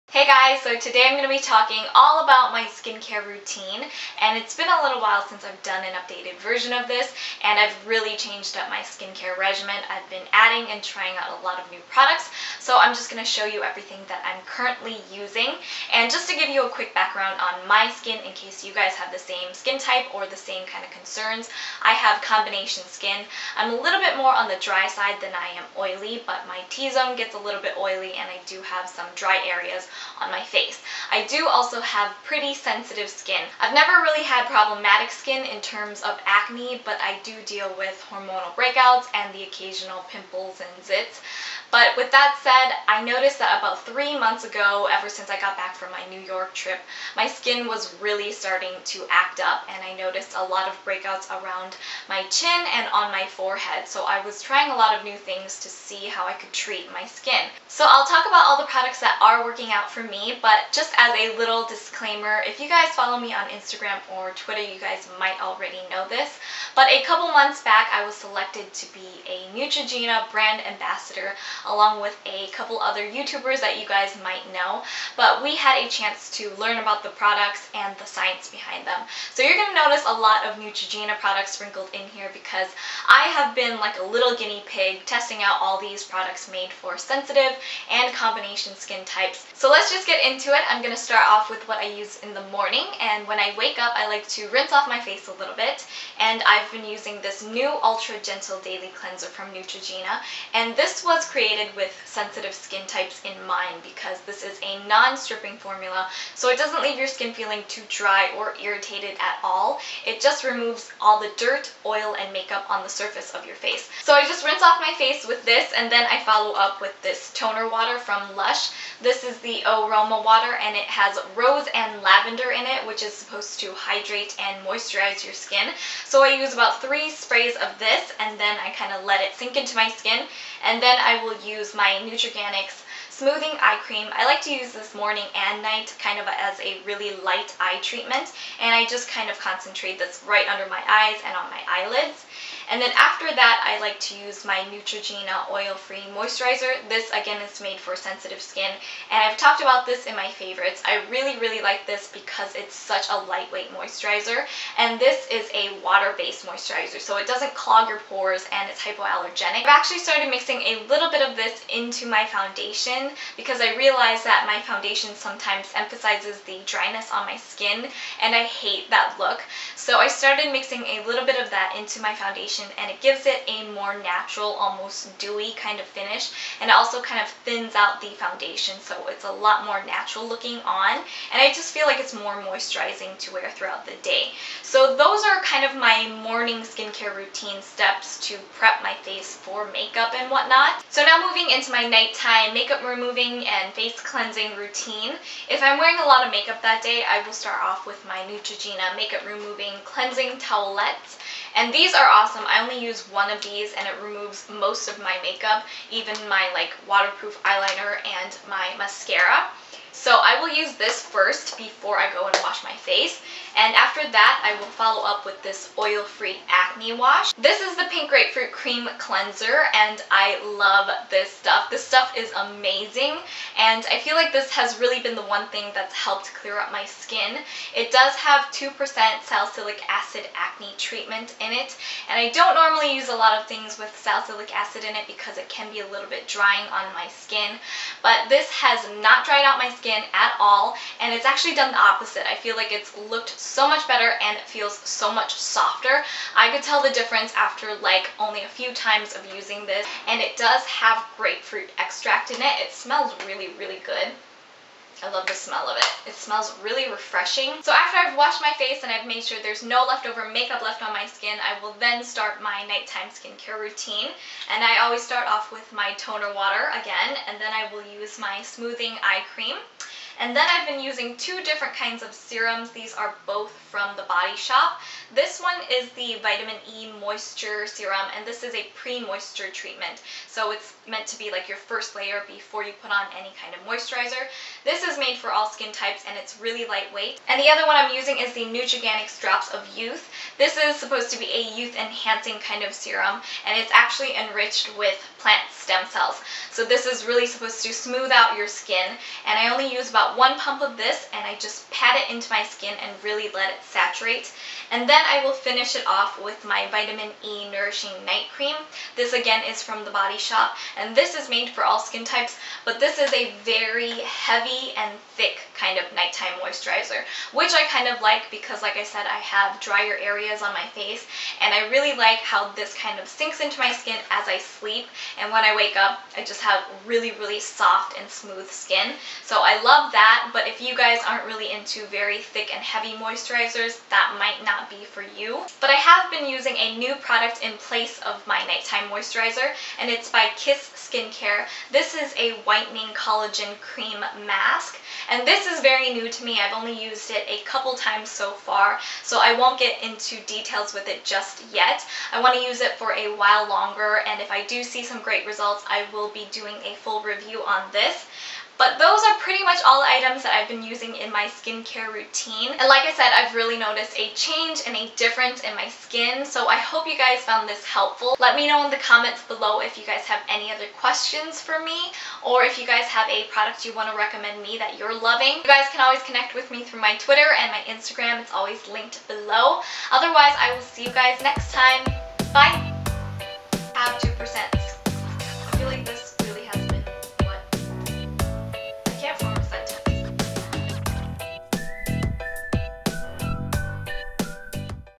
Current Skin Care Routine | LeSassafras
This is a long overdue skin care routine update. I've really changed up my skin care regiment so these are my current favorite skin care products.

I'm very excited to be selected as a Neutrogena Brand Ambassador! We had a chance to learn about the brand and the science behind their products. We even had a chance to sit down with a dermatologist to talk about our skin concerns. These products were most suitable for my skin type.

Ultra Gentle Daily Cleanser, Neutrogena
Eau Roma Water, LUSH
Nutriganics smoothing eye cream, The Body Shop
Oil-free Moisturizer, Neutrogena
Makeup Removing Cleansing Towelettes, Neutrogena
Oil-free Acne Wash, Neutrogena
Vitamin E Moisture Serum, The Body Shop
Nutriganics Drops of Youth, The Body Shop
Vitamin E Nourishing Night Cream, The Body Shop
Whitening Collagen Cream Mask, Kiss Skincare

L U V O C R A C Y